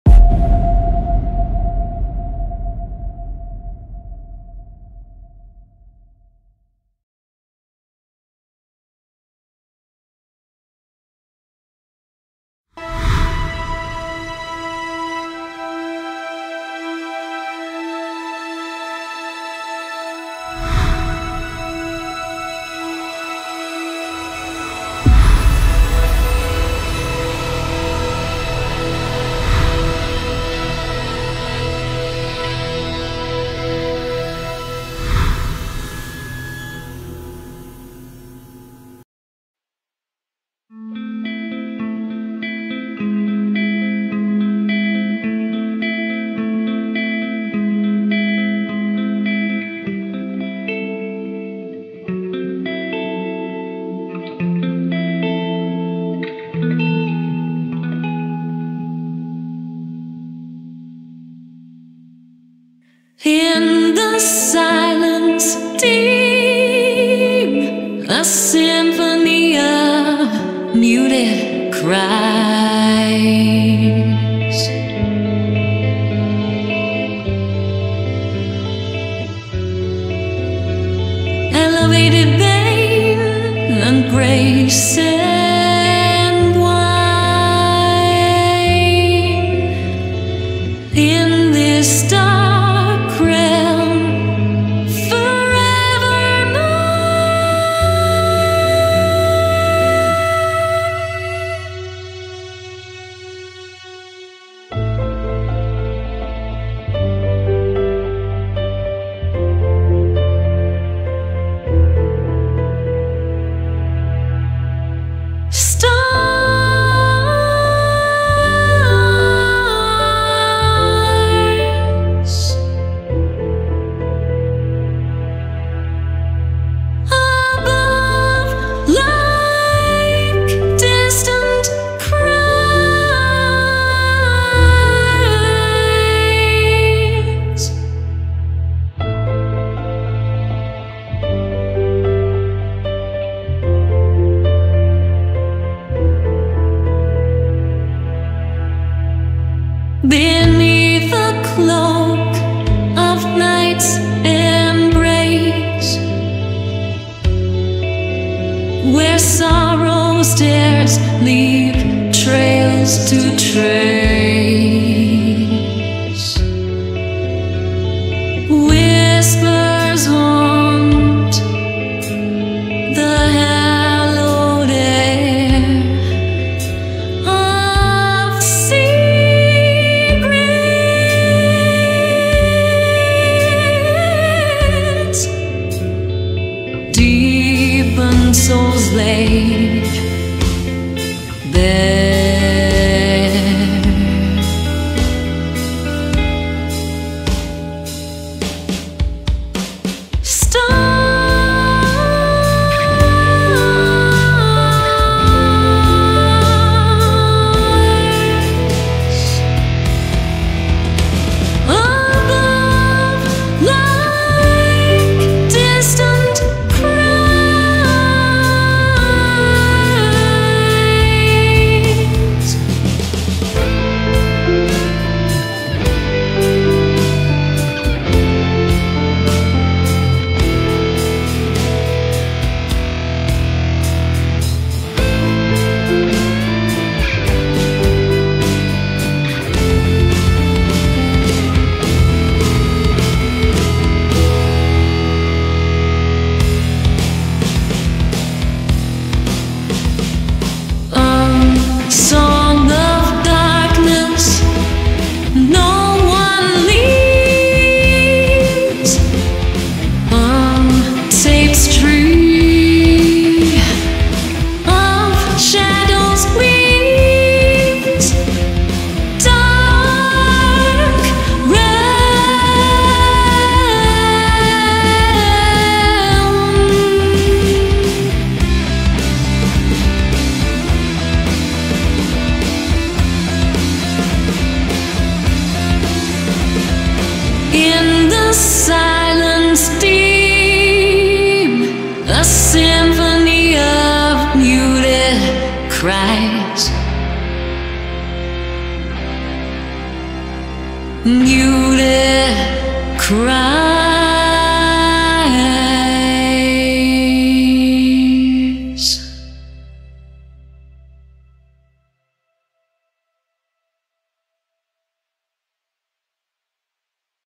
Dark Realm | Cinematic AI story Runway GEN-2 | Synthesizer V AI Natalie
DARK REALM   Cinematic AI story Runway GEN-2

Animation Generation: Runway Gen-2
Synthesizer V AI Natalie voice

A long time ago, a queen, in order to preserve her beauty and attain eternal life, decided to make a pact with the devil. As a consequence, the doors of darkness opened, plunging the kingdom into chaos..."